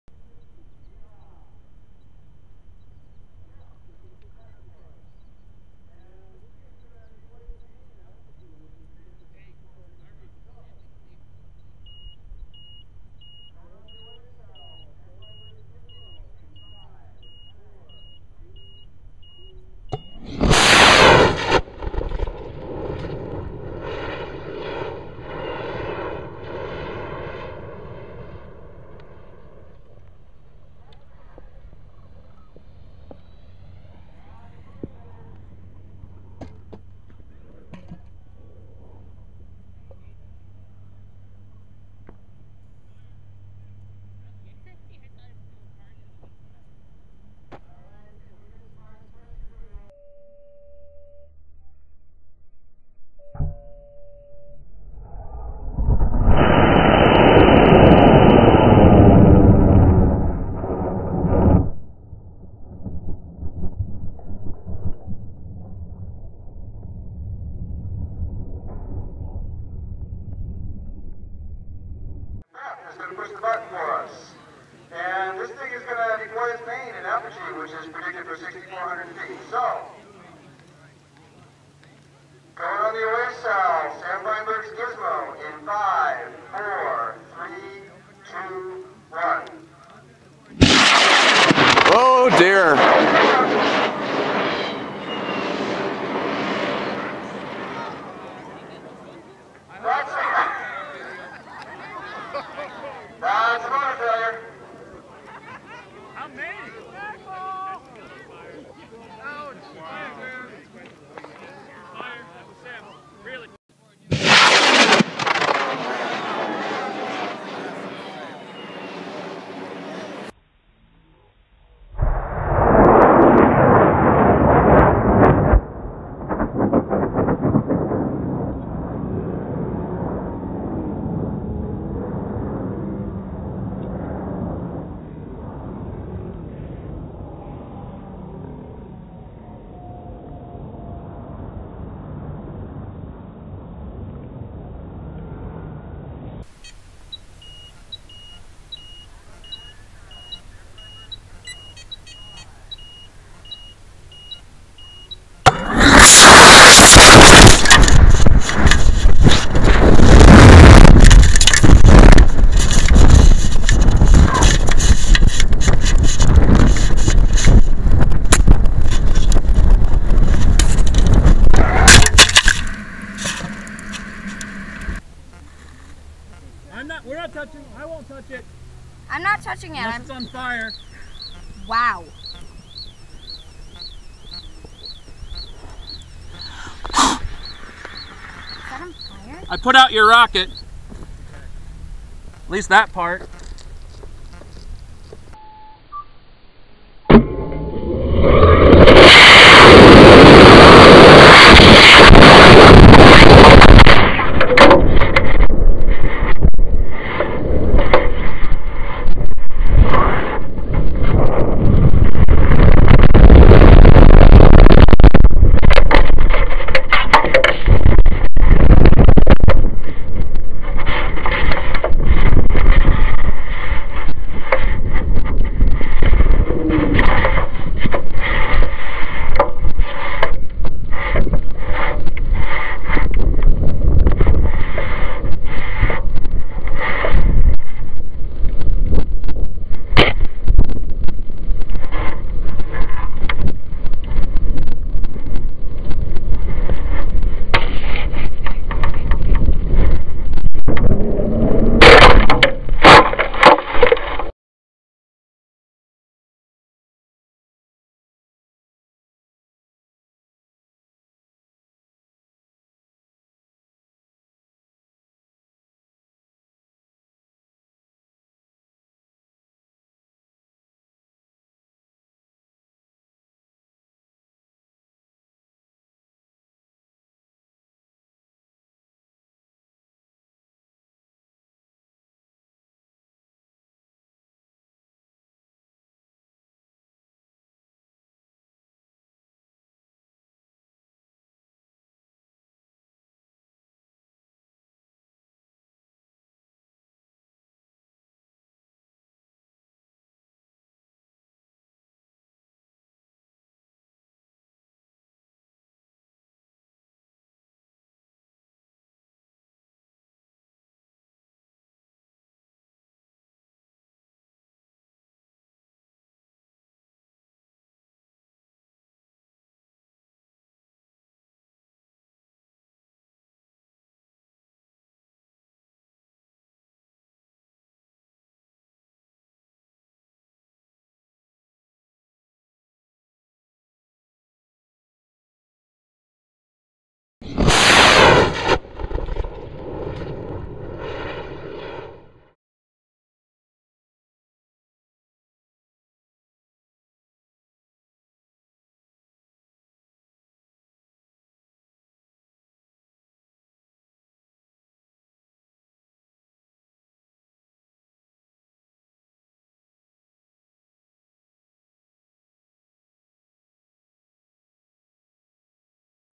L3 or Shred - Sam's NAR L3 Certification Attempt
Flight of Sam's Gizmo XL on a CTI M2250 and the resulting CATO (catastrophic motor failure).  The motor failed due to a weak liner which caused the aft (nozzle) end of the casing to burn through.  The casing and motor are being replaced by Cesaroni under warranty.  Needless to say, Sam is still an L2 for now.  This occurred at the LUNAR Snow Ranch launch on March 3, 2012.  Safety procedures all worked, no one was hurt, and the only damage was to the motor and the rocket.  The video and photos were taken by the rocket's creator as well as as some of the launch spectators.